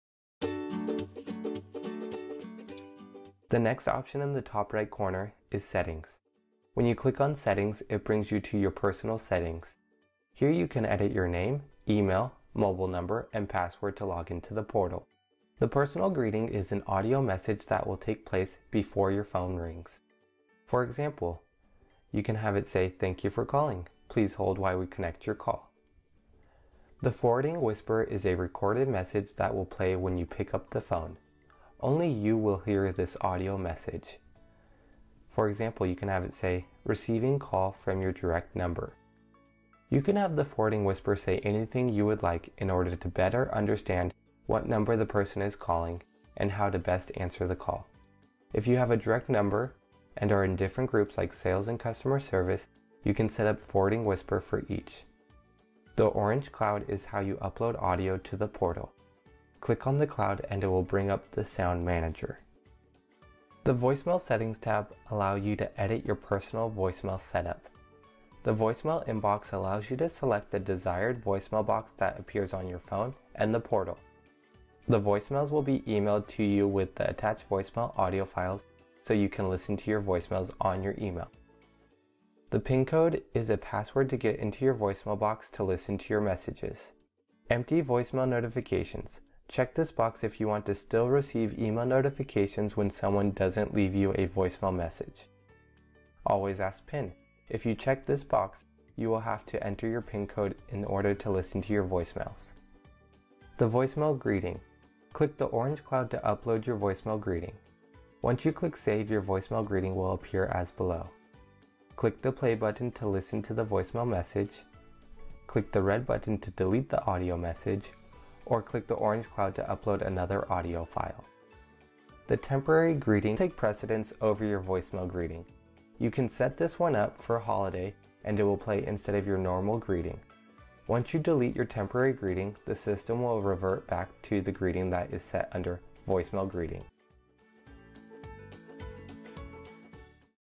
Managing Your Personal Settings on b-hive
Broadvoice b-hive platform allows users to change their personal settings directly from our online portal. Easily update your voicemail settings, create a welcome message, forwarding whisper, change your login information, and so much more.